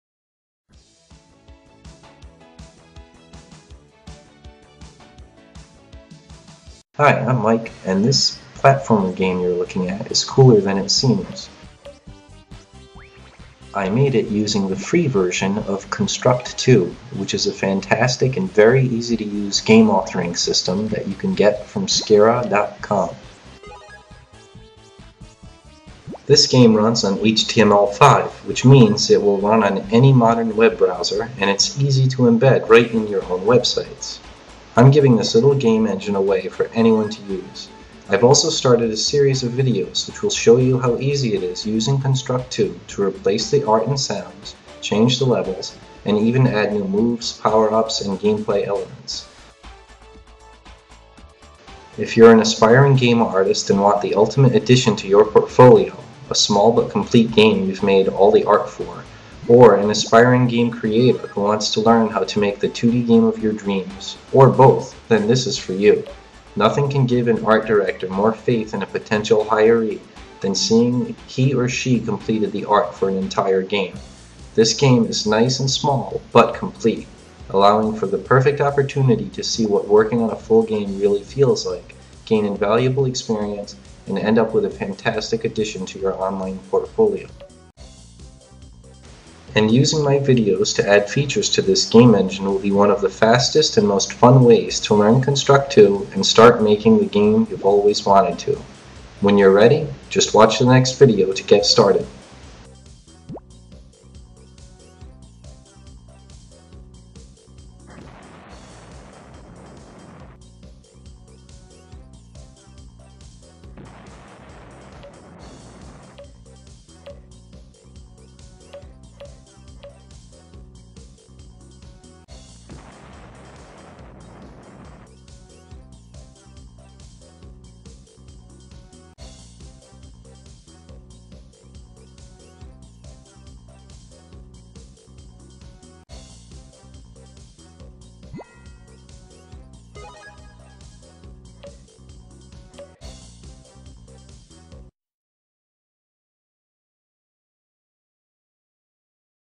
intro skin Platformer
Introduction to my free basic 2d platformer game engine which was made using the free version of Construct 2 Here are the links you'll need to follow along

and then unzip that plug-in file and put it in the path: Construct 2\exporters\html5\plugins

Have fun!

To learn about Spriter and support my tutorial videos please